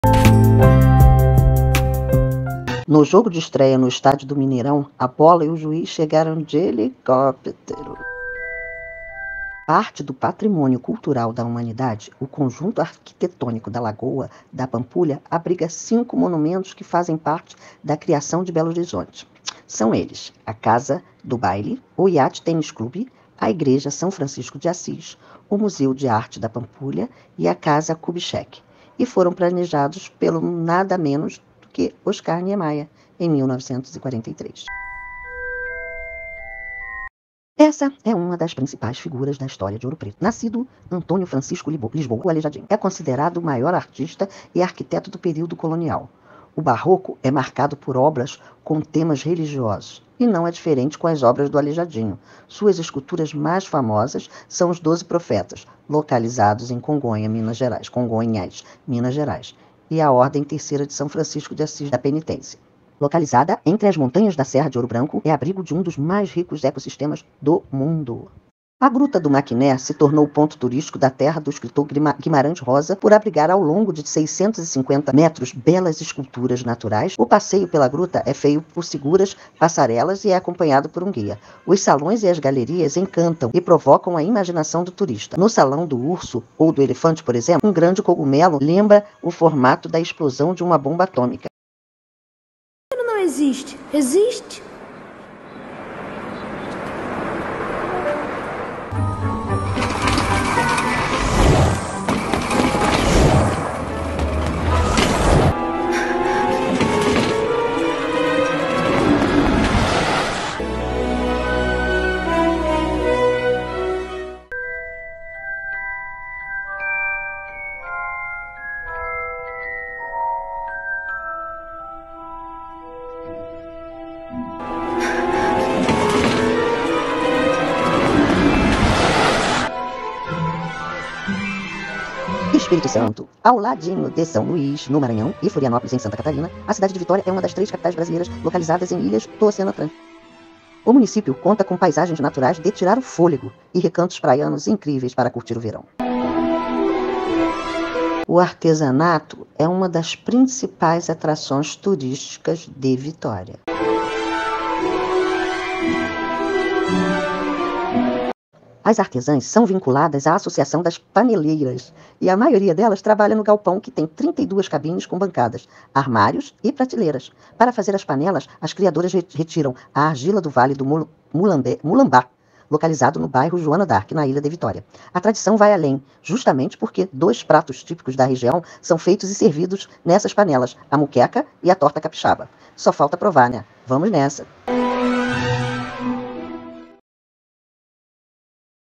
CURIOSIDADES MGxES + TRÊM do HARY POTTER no BRASIL
Veja como chegamos até a lua indo á Gramado no Space Adventure.

😍 PÁGINA DO FACEBOOK:      / mnexpedicoeseeventosoficial